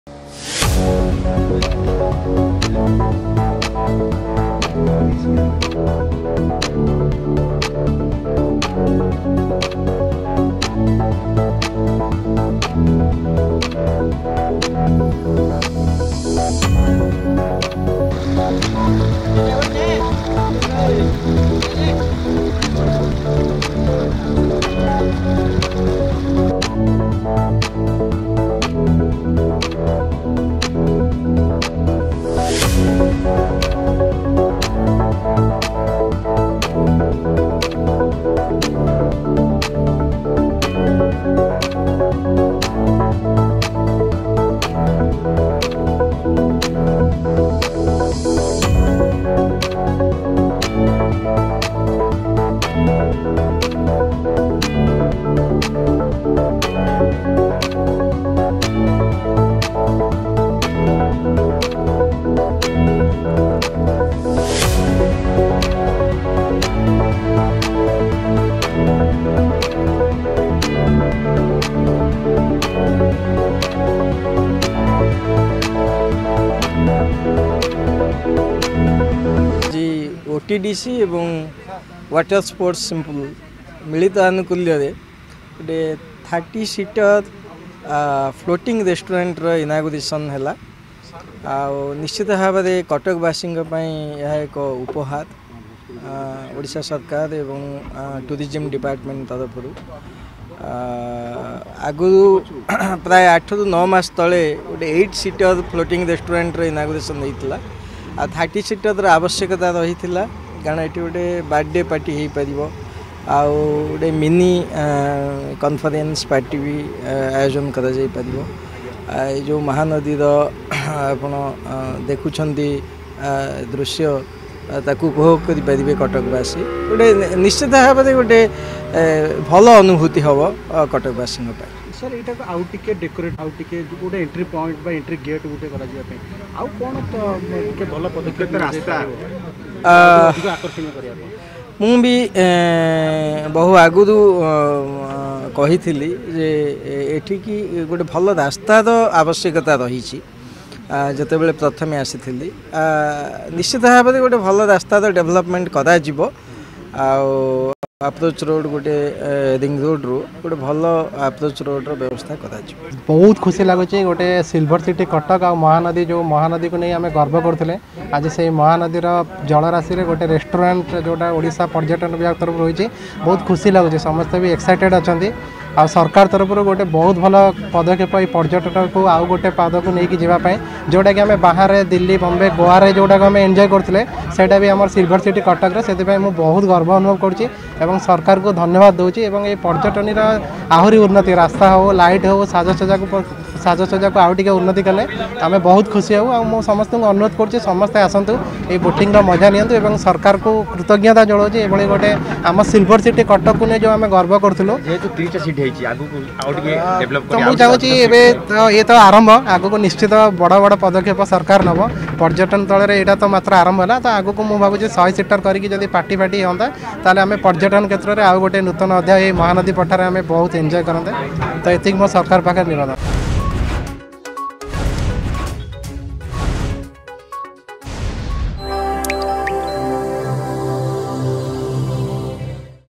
ମହାନଦୀରେ ଉଦଘାଟନ ହେଲା ଦ୍ଵିତୀୟ ଭାସମାନ ରେଷ୍ଟୁରାଣ୍ଟ
ମହାନଦୀର ଗଭୀର ଜଳ ରାଶିରେ ବୋଟିଂ ସହ ଫ୍ଲୋଟିଂ ରେଷ୍ଟୁରାଣ୍ଟର ମଜା ନେବେ ପର୍ଯ୍ୟଟକ । କିଛି ଏଭଳି ଏକ ନୂଆ ପ୍ରୟାସ ଆରମ୍ଭ କରିଛି ଏକ ଘରୋଇ ସଂସ୍ଥା water ସ୍ପୋର୍ଟସ ସିଂପ୍ଲେଲ  ଓ  ଓ.ଟି.ଡି.ସିର ମିଳିତ ଆନୁକୂଲ୍ୟରେ  ମହାନଦୀରେ ଉଦଘାଟନ ହେଲା ଦ୍ଵିତୀୟ ଭାସମାନ ତିରିଶ ସଂଖ୍ୟା ଵିଶିଷ୍ଟ ରେଷ୍ଟୁରାଣ୍ଟ